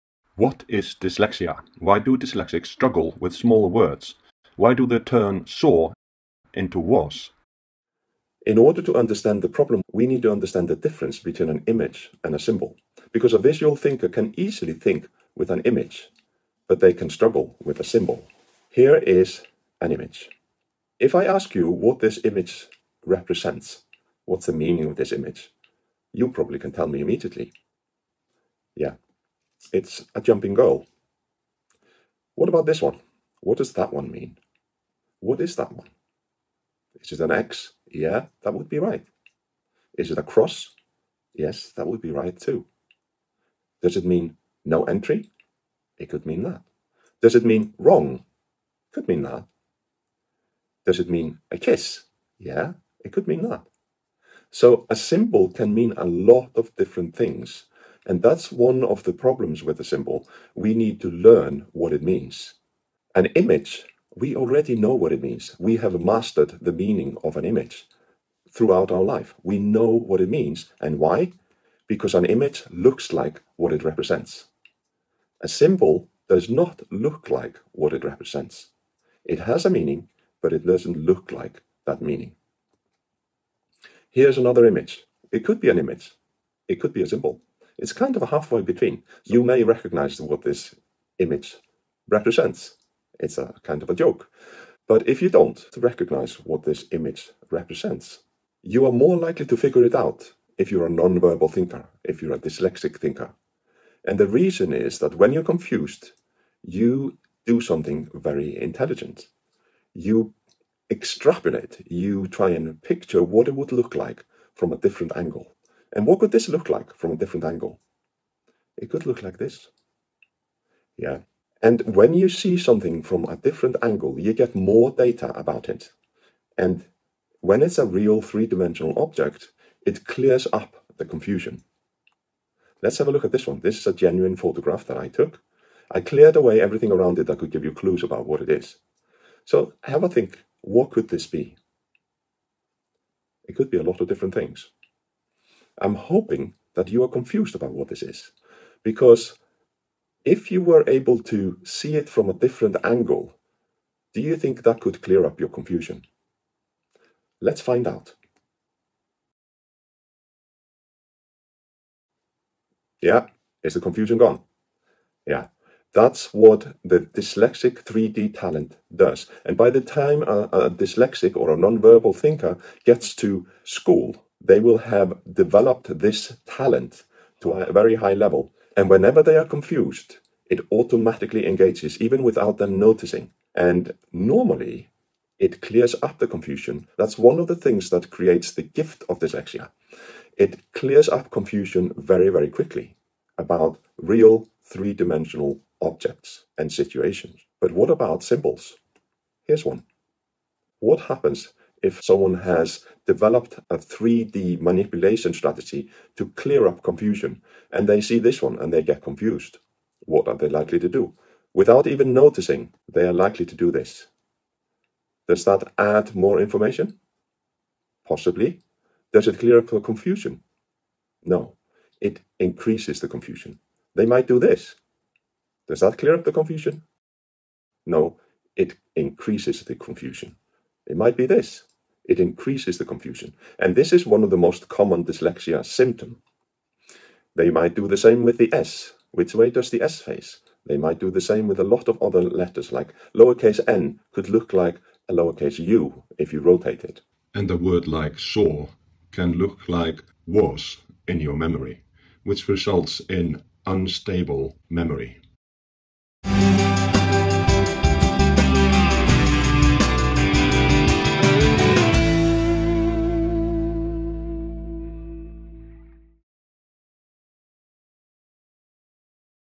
What is dyslexia? Why do dyslexics flip words and letters?
The reason lies in a perceptual talent, which is the gift of dyslexic thinking.
This talent results in many advantages, but causes problems with the written word because they are 2-dimensional symbols.

Here we explain why this problem exists.

The solution is very simple; we need to stop the dyslexic from turning the 2D symbols into 3D images in the mind, and we can do that by eliminating the CONFUSION.

This can be done in as little as 5 day in a Davis Dyslexia Correction Programme, resulting in a minimum of 2-year jump in reading ability.

If this is done early enough (age 5-6), then the problems of dyslexia can be PREVENTED.